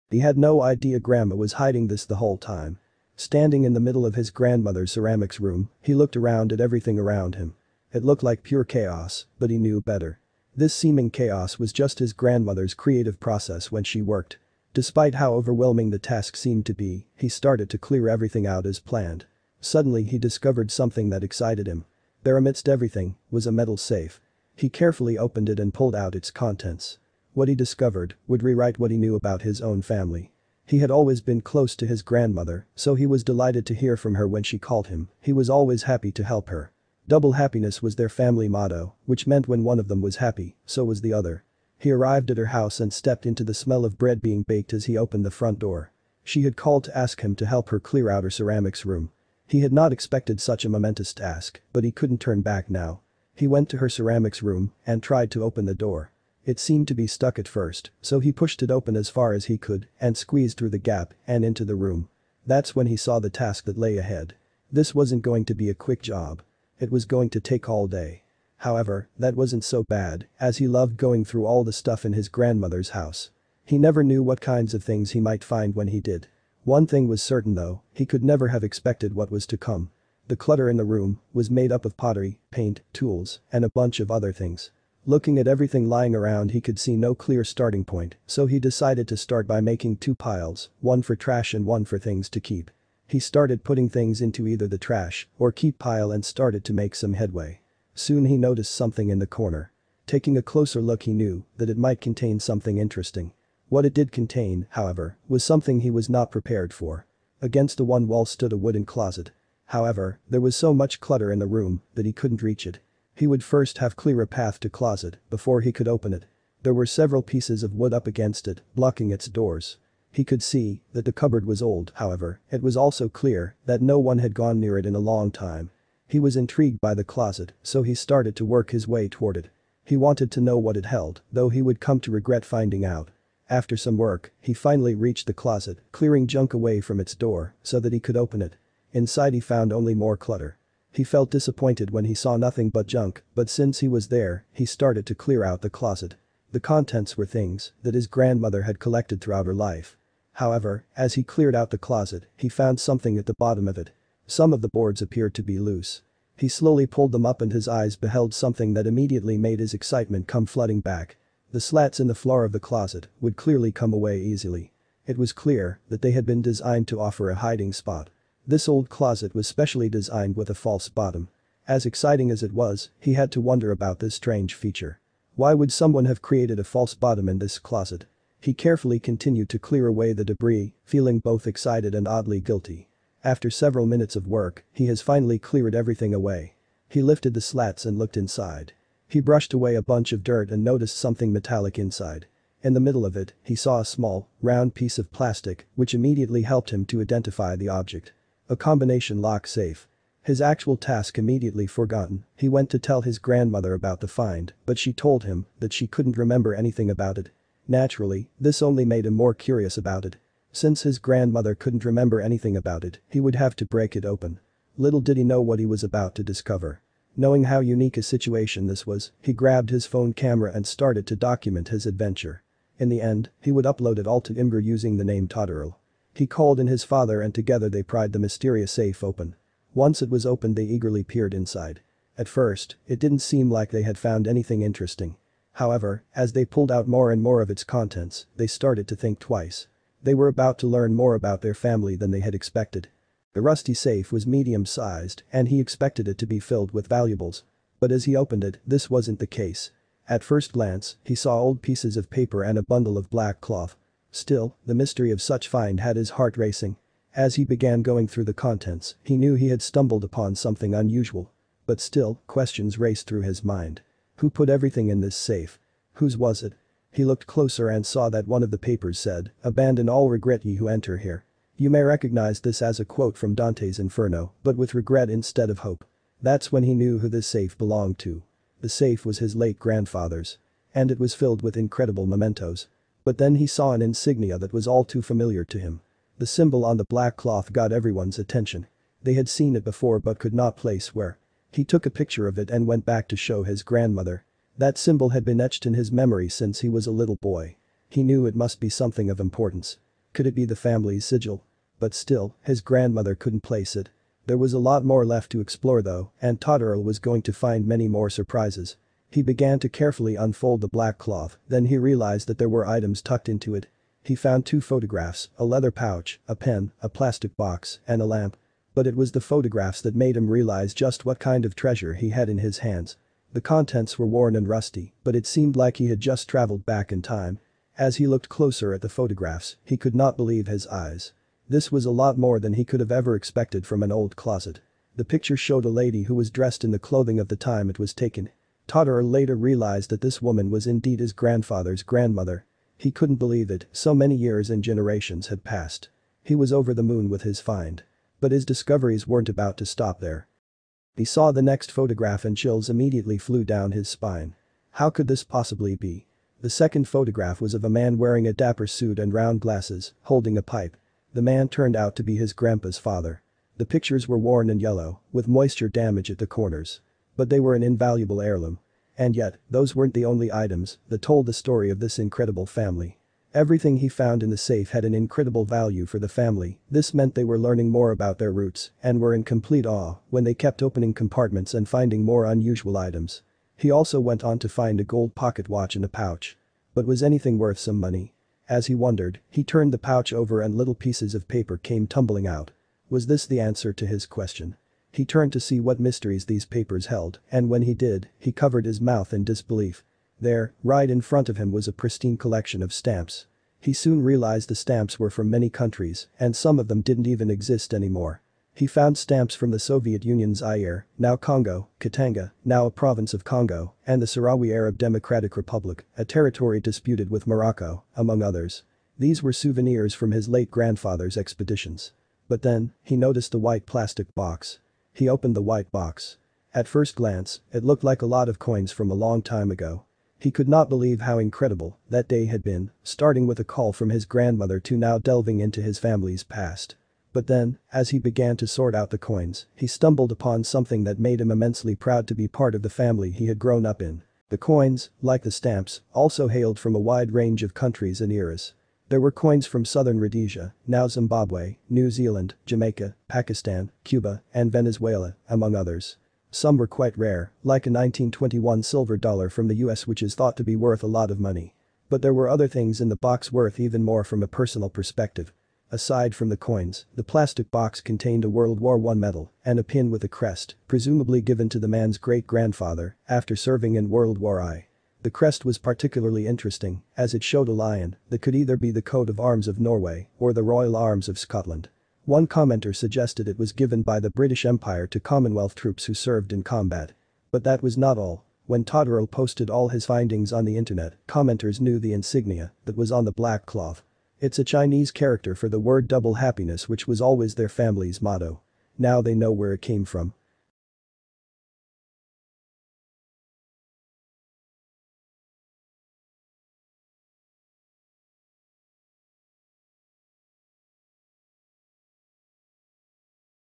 He Had No Idea Grandma Was Hiding This The Whole Time
Standing in the middle of his grandmother’s ceramics room, he looked around at everything around him. It looked like pure chaos but he knew better. This seeming chaos was just his grandmother’s creative process when she worked. Despite how overwhelming the task seemed to be, he started to clear everything out as planned. Suddenly he discovered something that excited him. There amidst everything was a metal safe. He carefully opened it and pulled out its contents. What he discovered would rewrite what he knew about his own family.